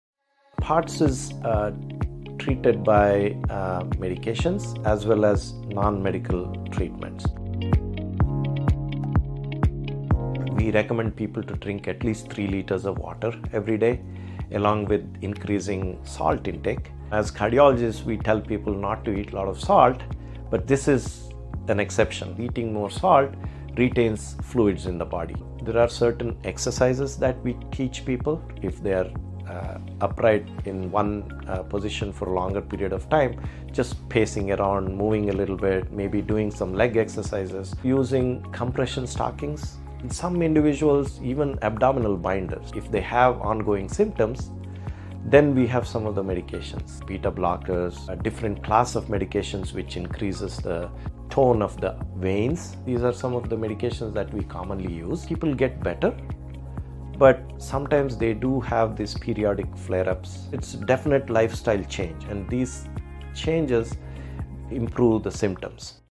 How it POTS Treated? ♥
Dr. Prakash Goutham, Cardiologist at OrlandoHealth Heart & Vascular, explains treatment for Postural Orthostatic Tachycardia Syndrome (POTS). Learn about the symptoms and causes of this often-misunderstood condition on the Content Hub